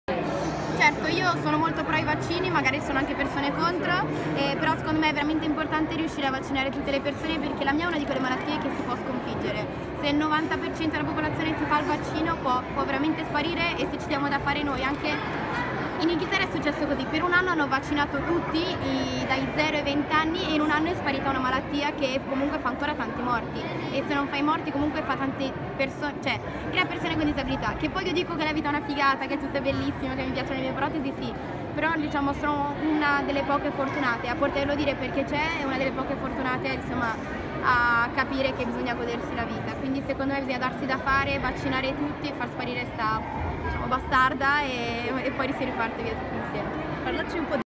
17 05 17 Bebe Vio sono pro vaccini sono importanti evitano morti e disabilita 0047 tv
«La mia è una di quelle malattie che si può sconfiggere, se il 90% della popolazione si facesse il vaccino la meningite potrebbe sparire» Bebe Vio in occasione della campagna educativa "Una vita da Social"